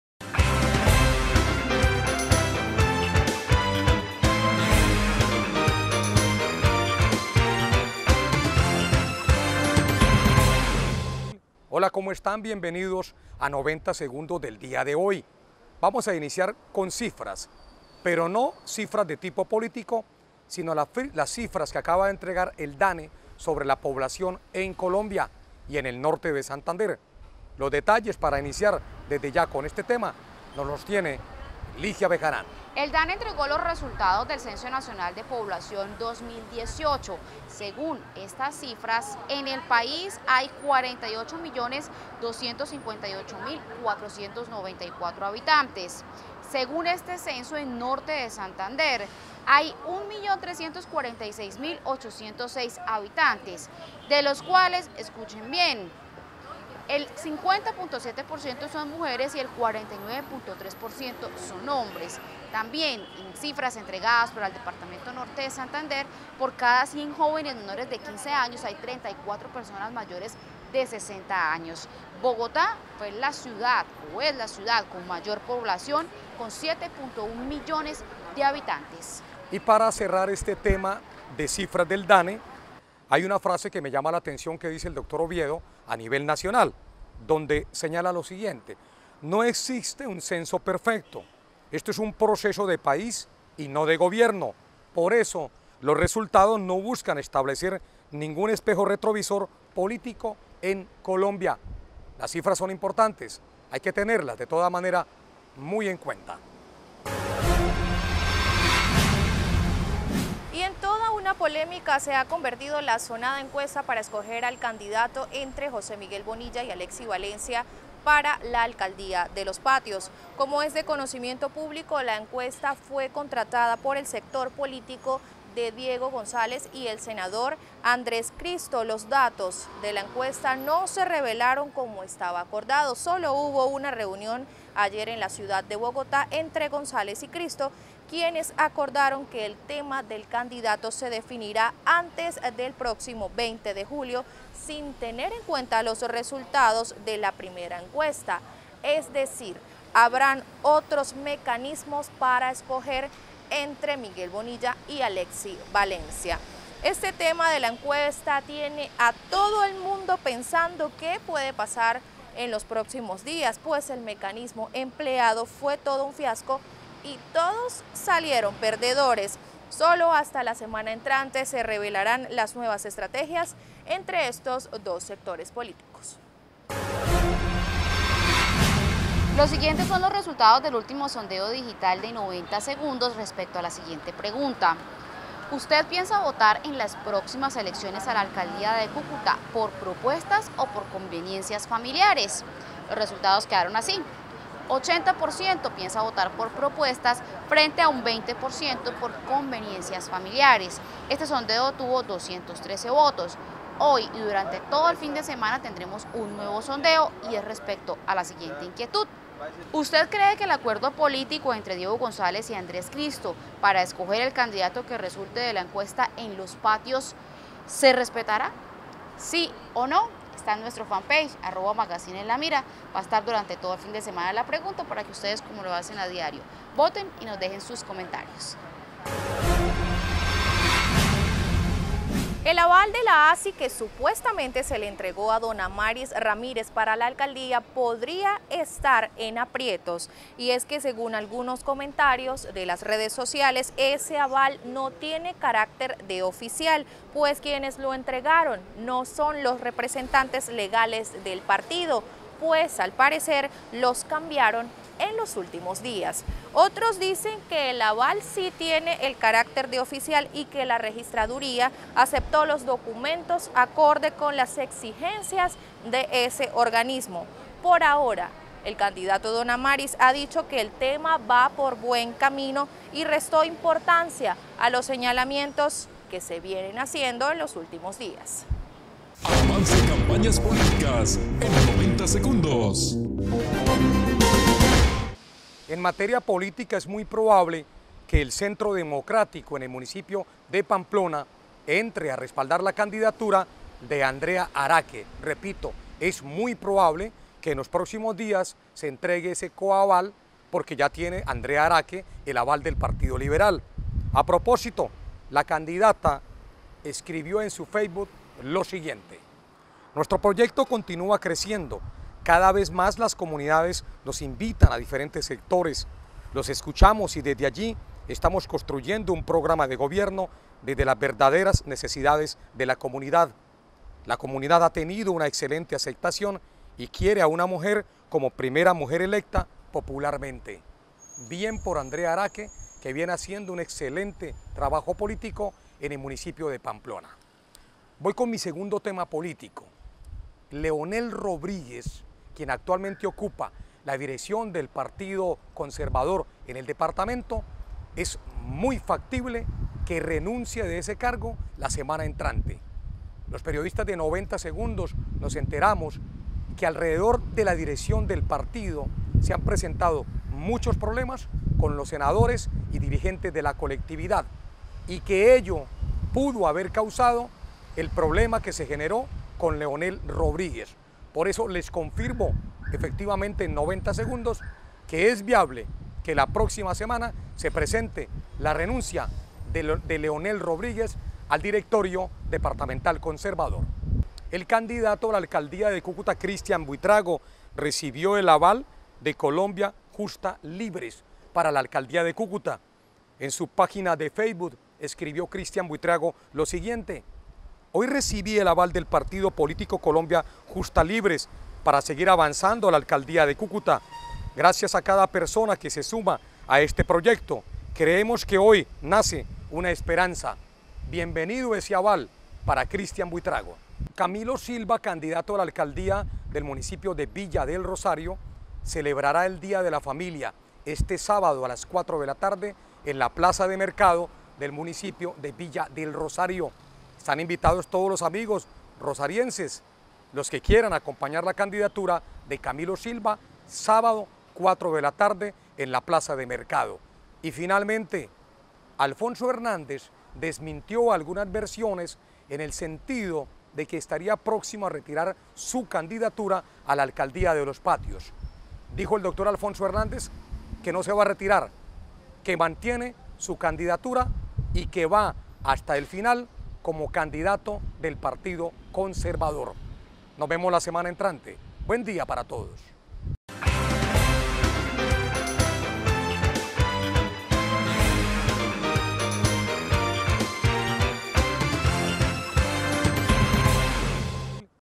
Renuncia Leonel Rodríguez del Directorio
Viernes

Con Fredy Fernando Parada Temas:

1. DANE presentó resultados del censo nacional de población
2. Polémica por encuesta en Los Patios
3. Sondeo de Opinión Magazín en la Mira
4. Aval Chimbo?
5. Andrea Araque sigue su campaña: Renuncia Leonel Rodríguez del Directorio
6. Alfonso Hernández no se retira: Camilo Silva en campaña: Cristian Buitrago recibe aval de Colombia Justa Libre